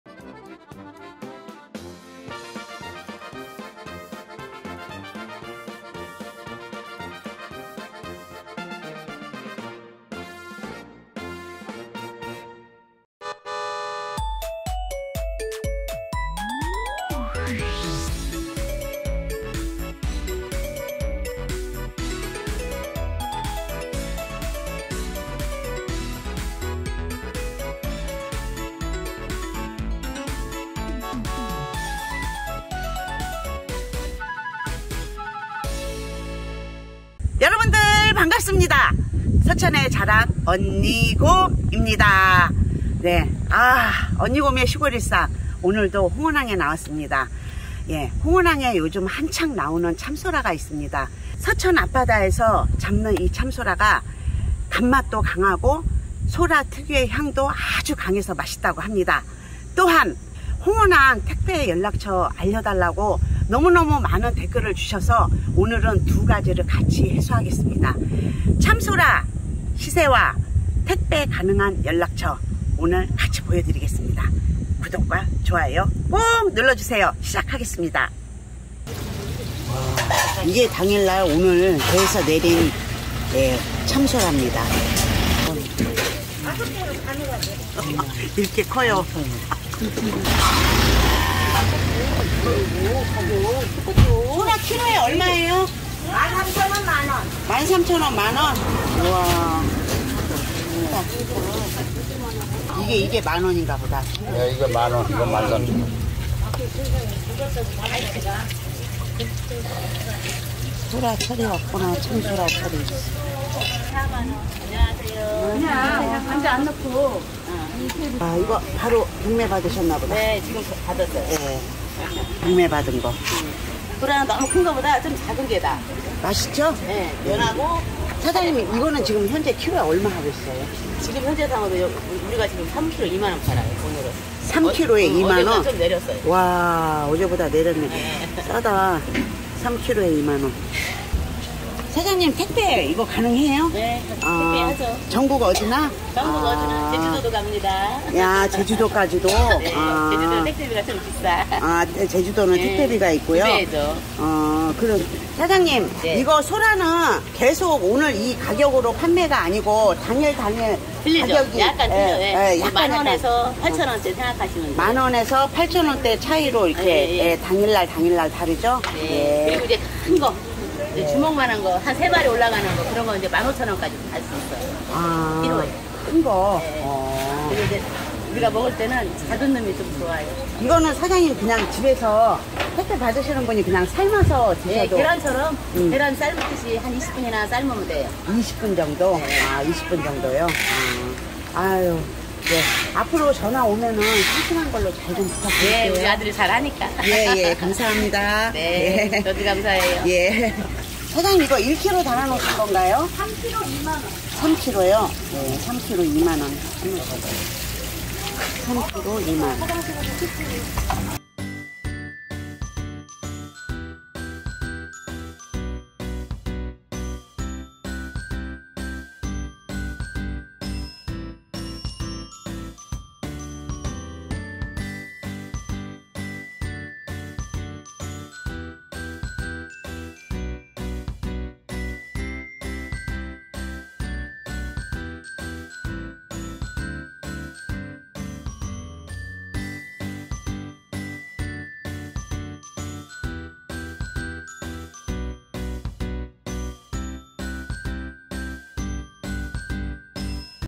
지금 홍원항은 참소라 풍년 소문듣고 전국 각지에서 온 사람들로 시끌벅적
지금 홍원항은 참소라가 풍년이라 소문듣고 전국 각지에서 온 사람들로 시끌벅적! 홍원항 택배 구매 연락처 공개